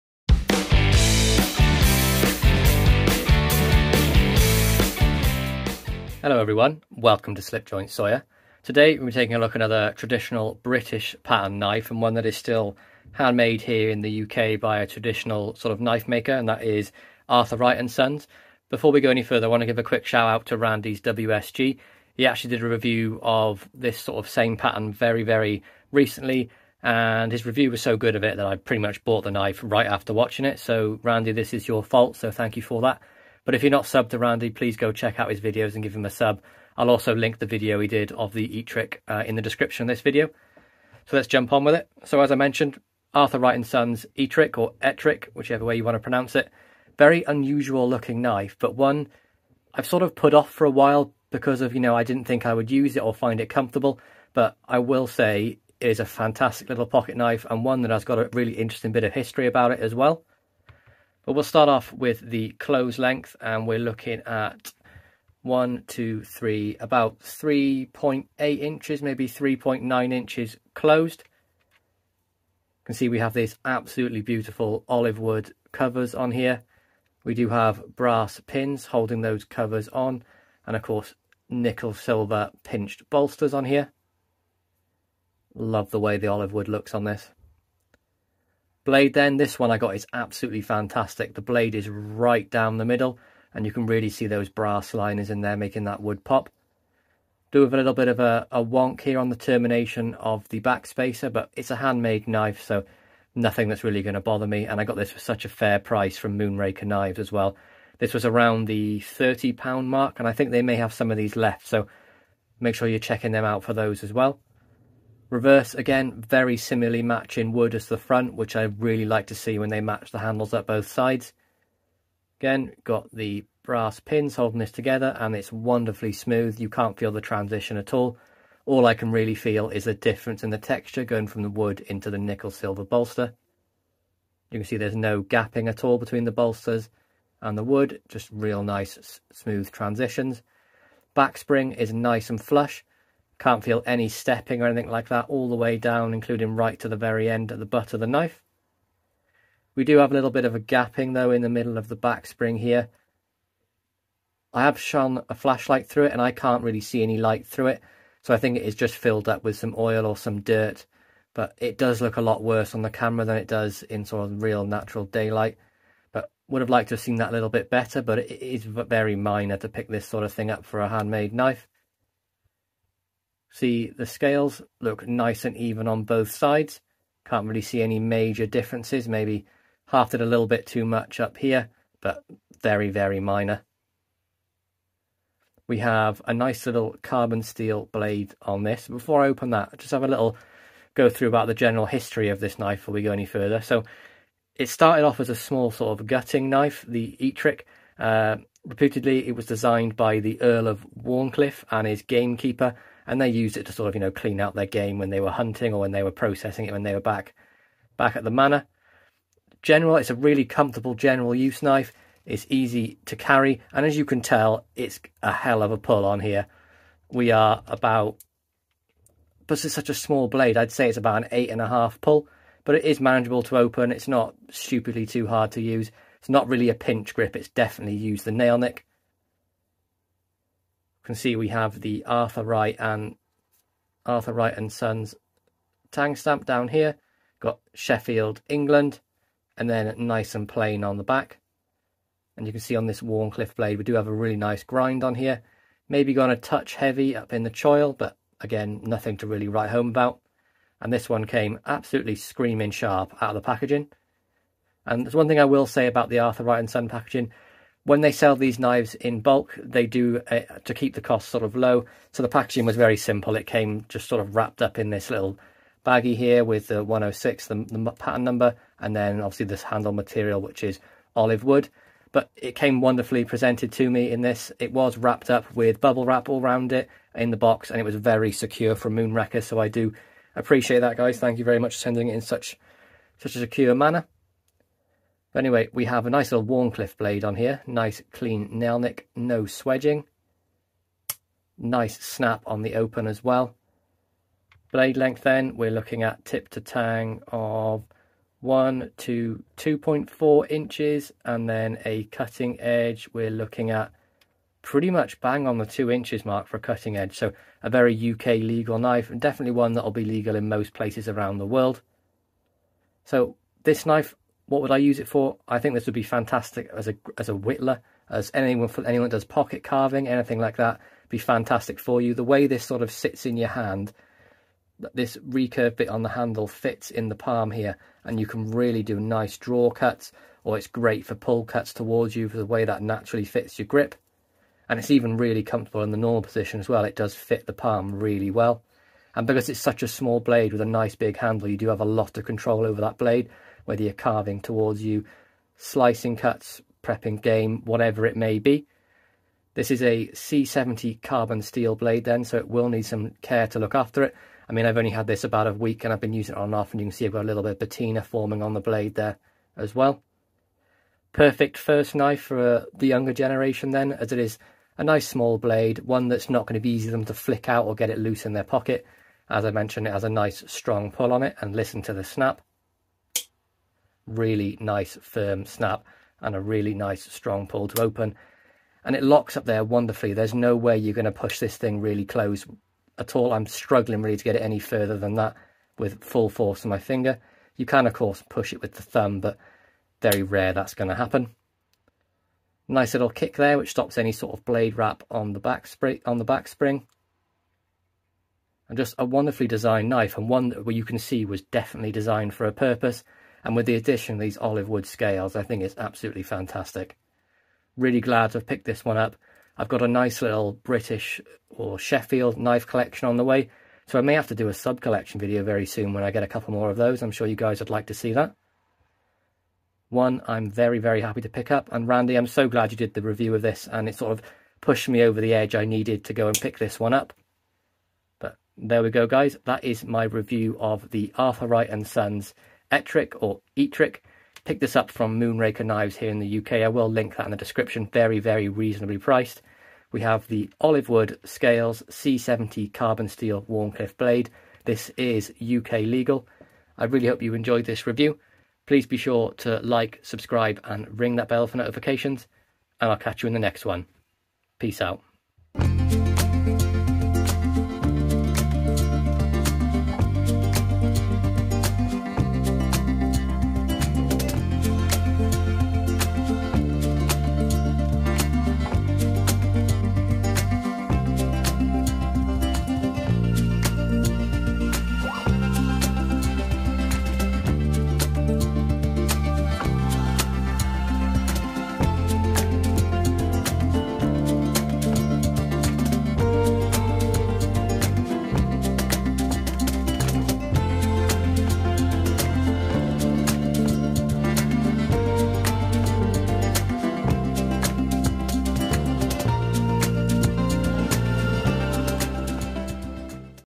Arthur Wright & Sons Ettrick Traditional Sheffield Pocket Knife
Review of the Arthur Wright & Sons Ettrick Traditional Sheffield Pocket Knife

Join myself and loads of other members of the Knife Community in the Slipjoint Sanitarium Discord chat

Slipjoint Sawyer
Creator\Reviewer